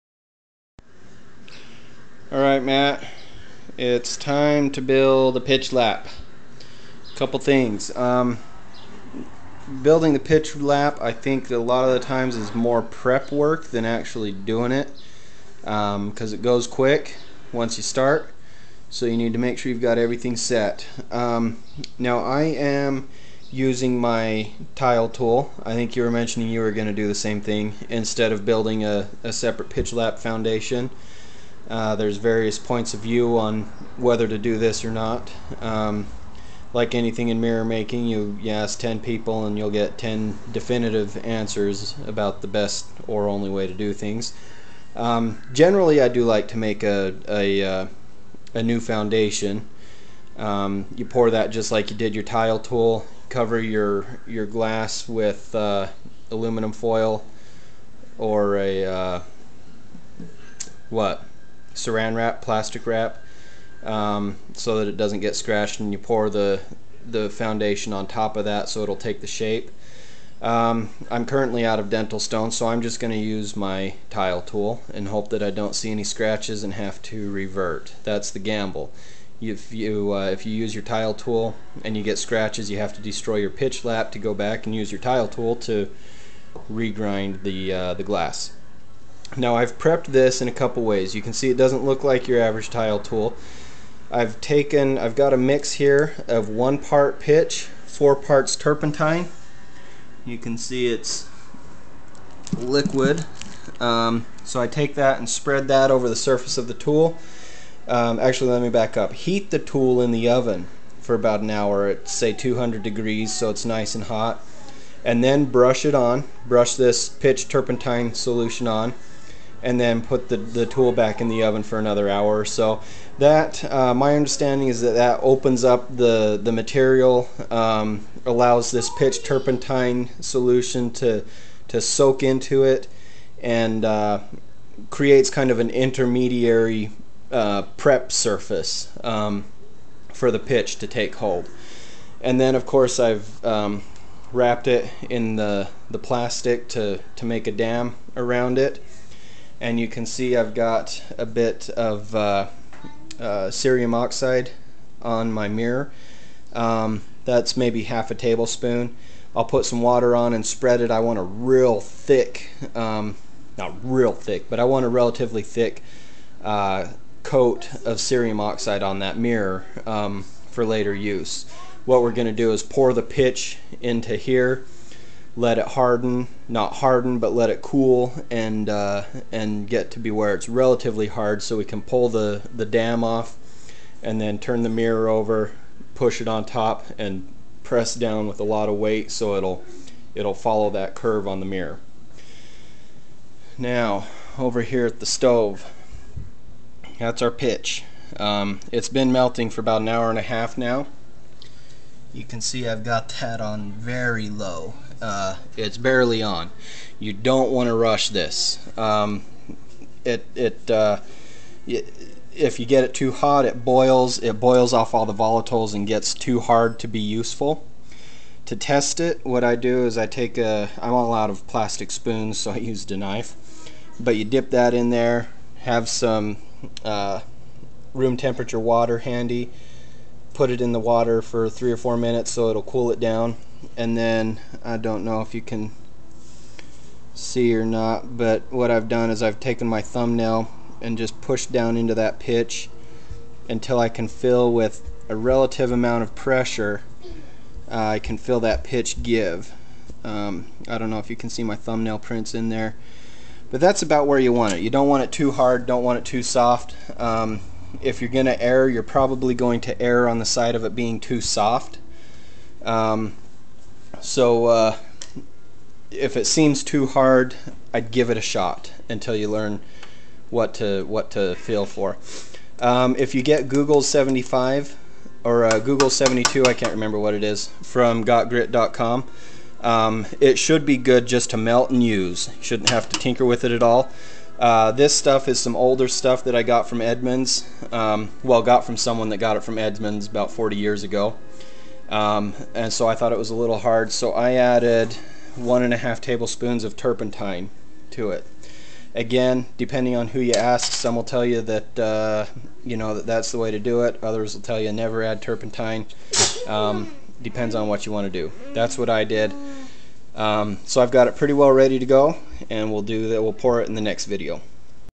Pouring the Pitch Lap - Part 1
This shows the prep for pouring your pitch lap to start polishing your telescope mirror. It's important to prepare properly because once you start working you need to keep at it until you're done -- There's no pausing in the middle of the process.